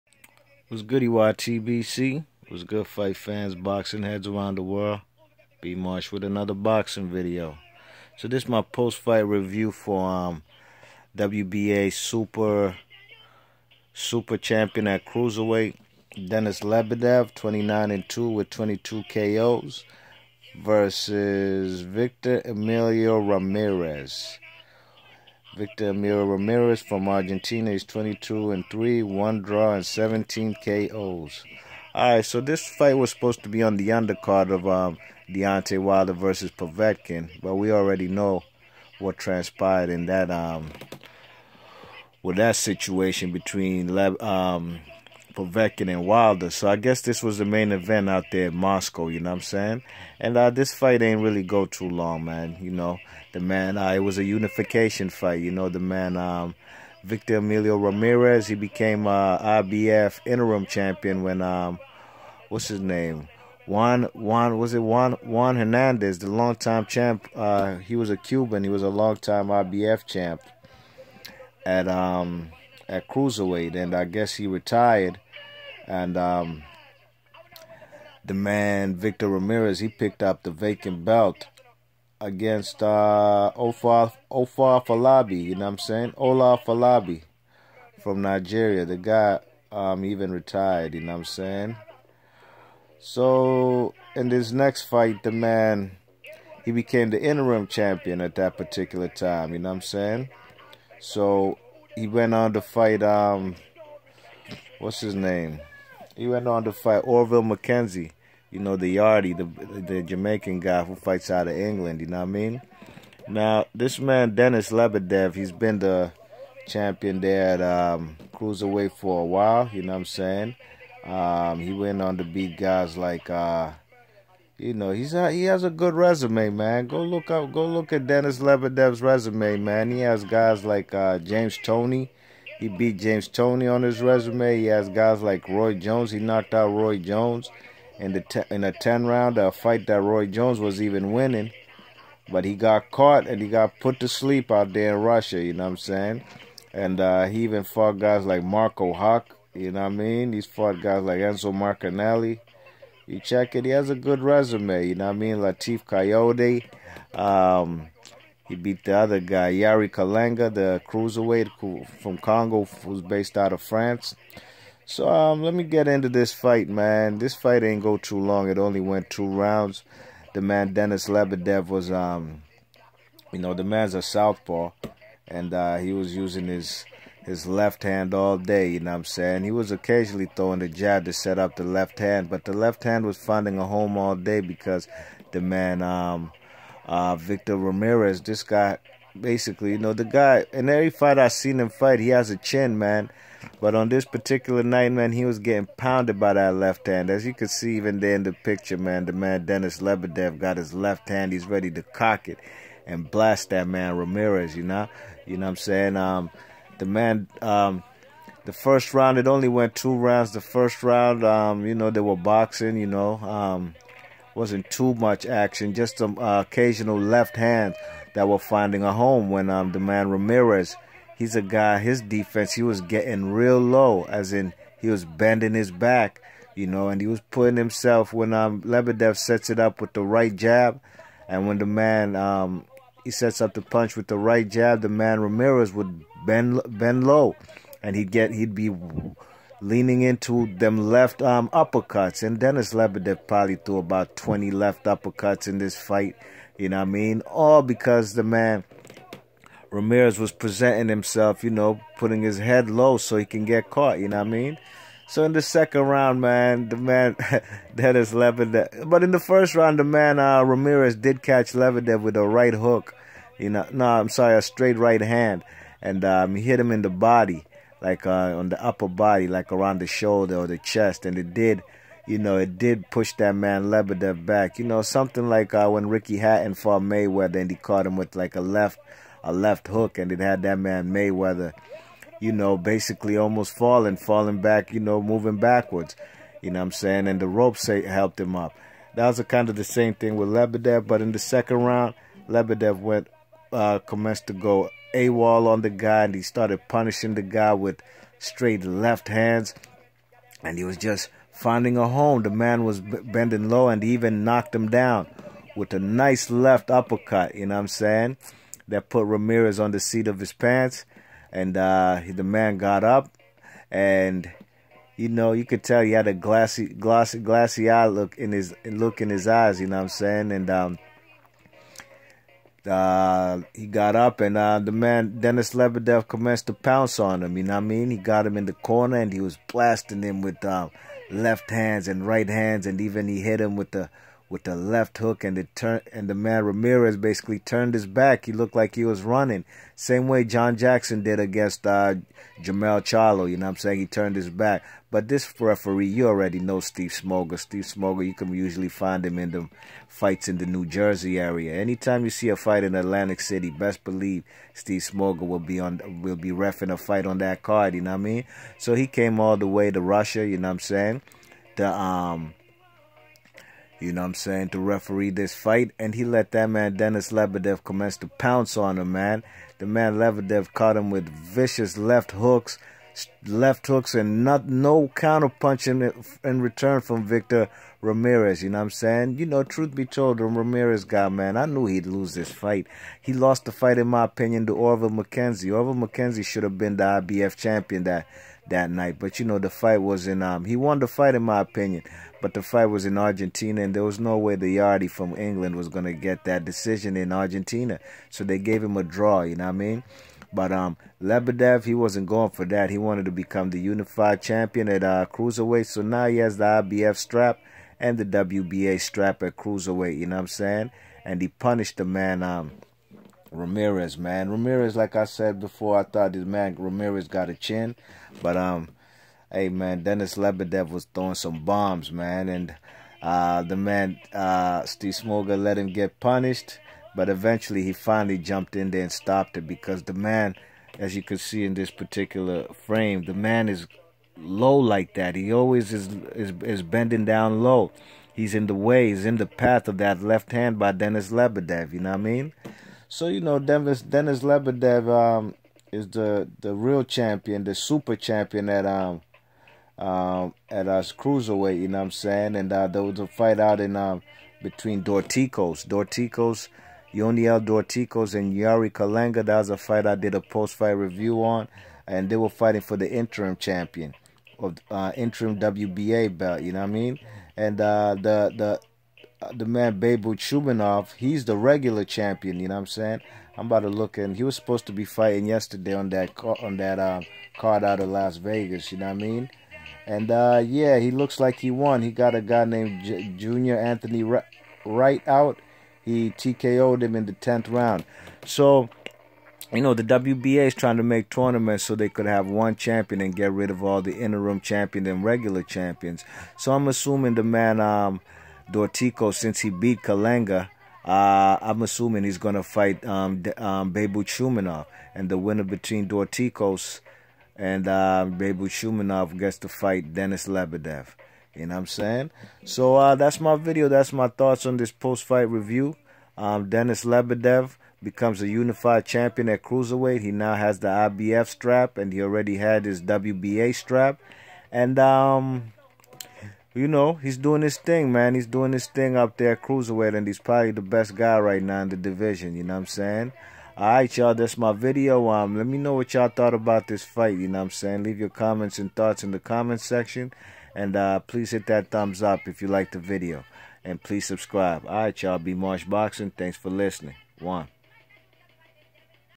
POST FIGHT REVIEW:DENIS LEBEDEV TKO'S VICTOR RAMIREZ TO UNIFY WBA/IBF CRUISERWEIGHT TITLES!
DENIS LEBEDEV BECOMES A UNIFIED WBA/IBF CRUISERWEIGHT CHAMP AFTER STOPPING ARGENTINIAN  VICTOR RAMIREZ VICIOUSLY IN 2 ROUNDS IN MOSCOW RUSSIA.